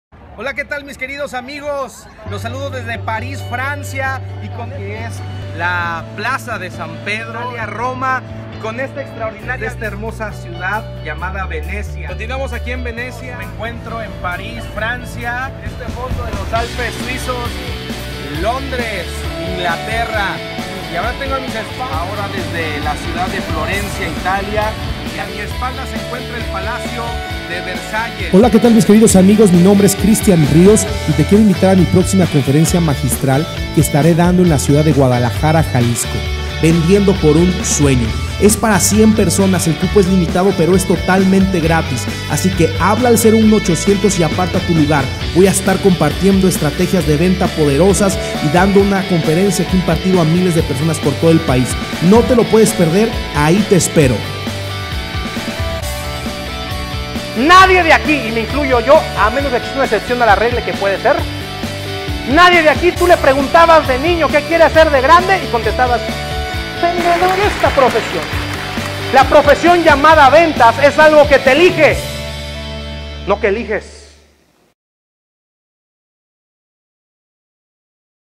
CONFE GUADALAJARA
Conferencia Magistral "Vendiendo por un Sueño" totalmente GRATIS en Guadalajara impartida por el Director Nacional de ICEMéxico el Lic. Christian Gabriel Rios Cupo limitado a 100 personas, próximo Sábado 20 de Febrero, aparta tu lugar ahora mismo llamando al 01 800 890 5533.